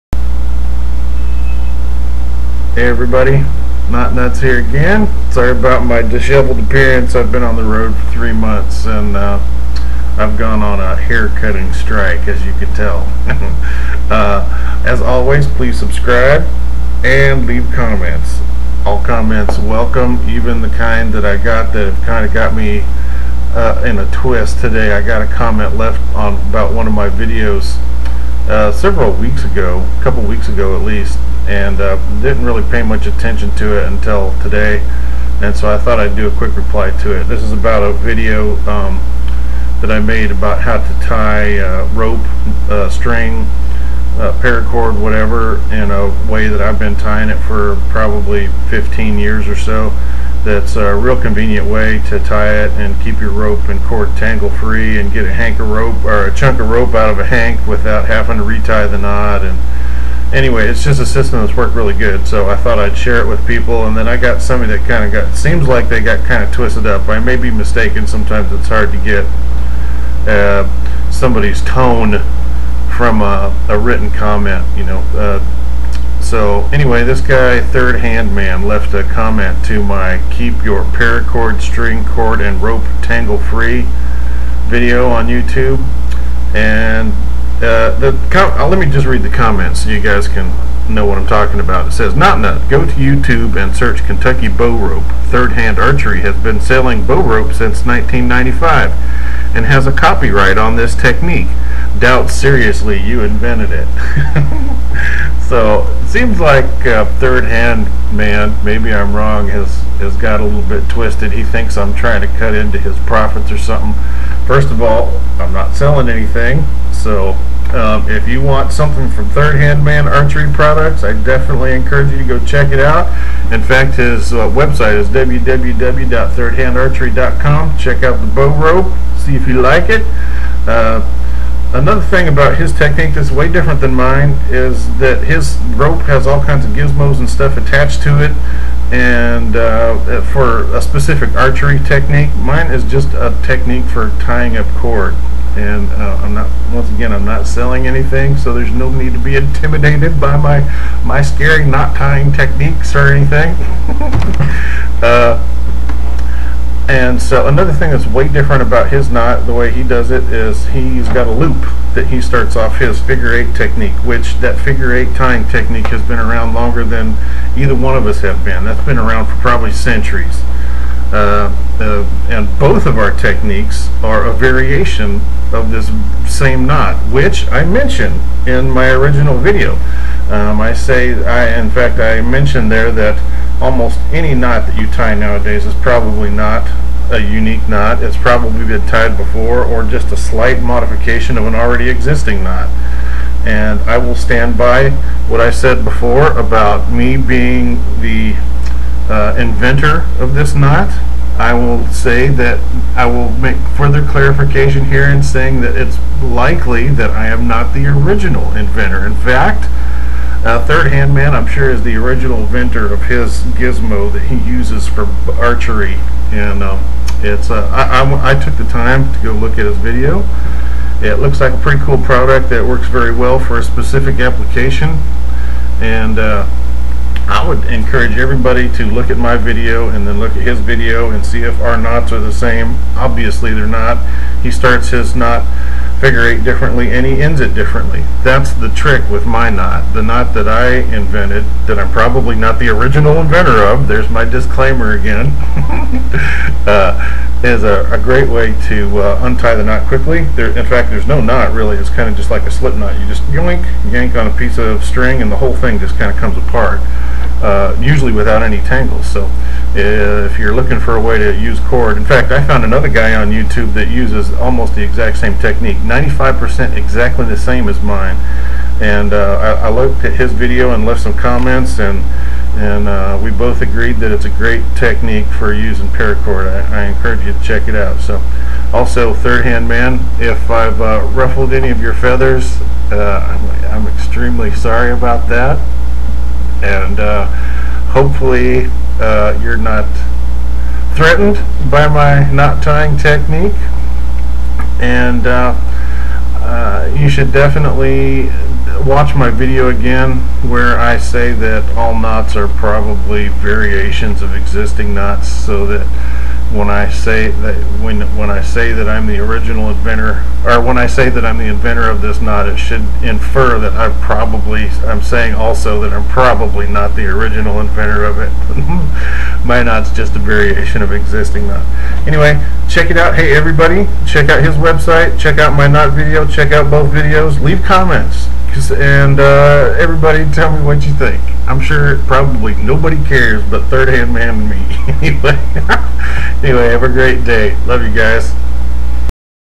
Video reply to Thirdhandman's comments to one of my knotting videos
This video is made in reply to thirdhandman's comment to my video entitled "Keep your Paracord, string, cord and rope tangle-free."

Thirdhandman's comments seemed a bit defensive and he even mentions "copy wright" in his video.  I'm not entirely sure why he would even do that unless it was a pale attempt to scare me.

Anyway, please check out thirdhandman's website about the Kentucky bow rope gizmo he sells at this